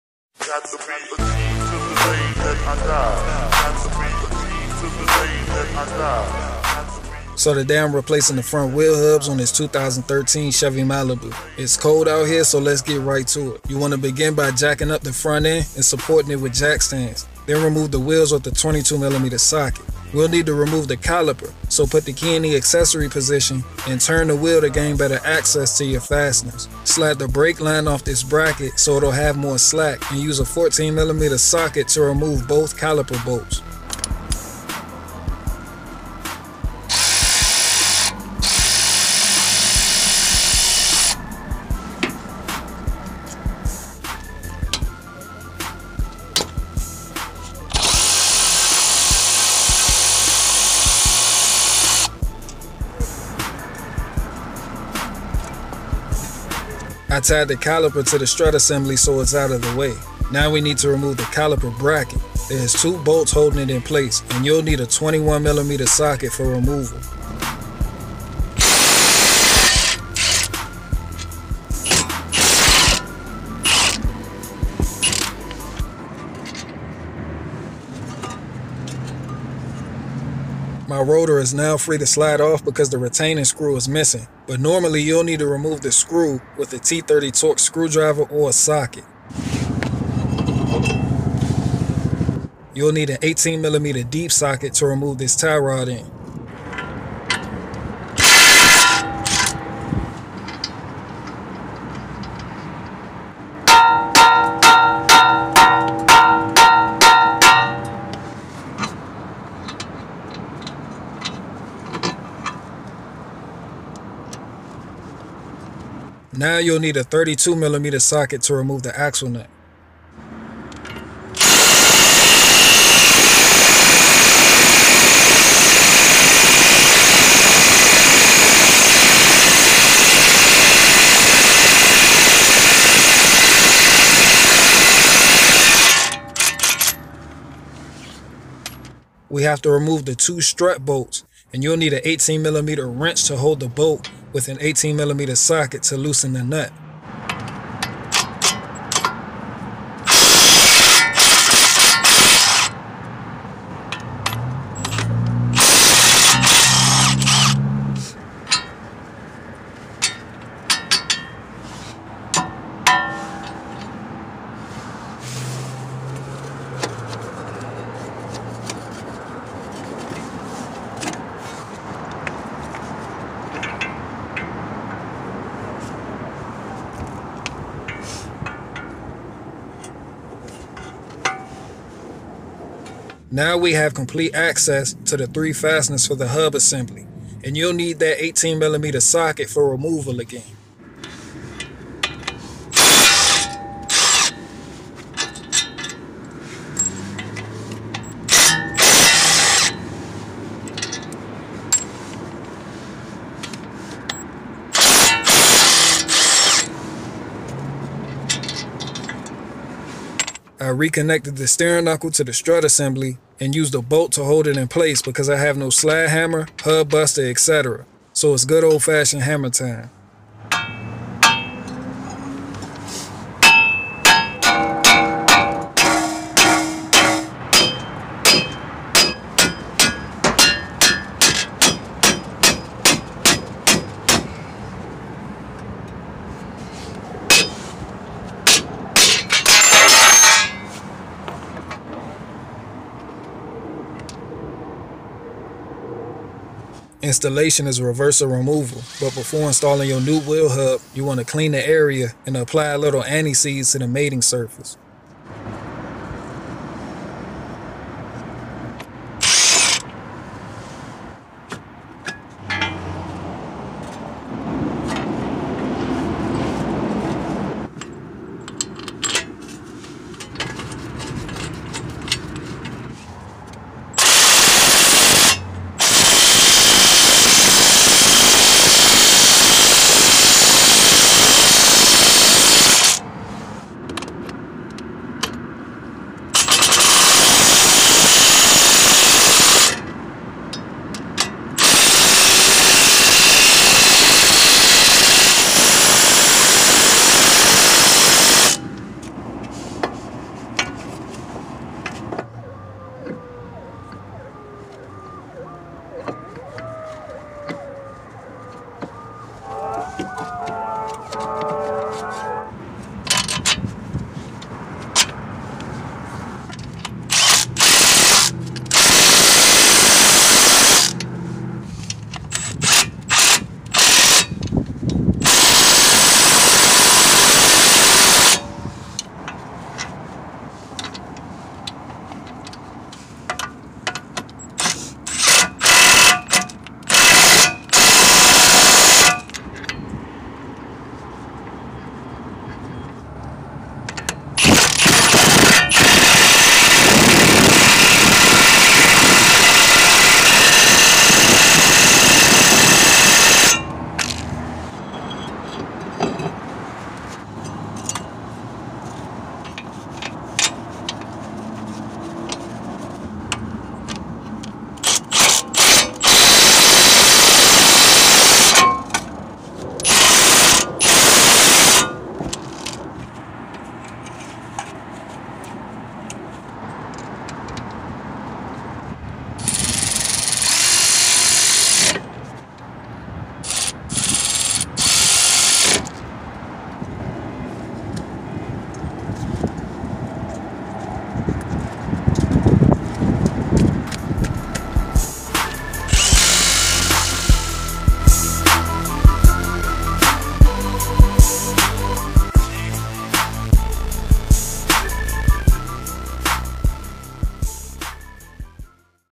FRONT WHEEL HUB REPLACEMENT (2013-2016 CHEVY MALIBU)
Replacing the front wheel hubs on a 2013 Chevy Impala
I assembled the torque specs from various locations so take them with a grain of salt and do your own research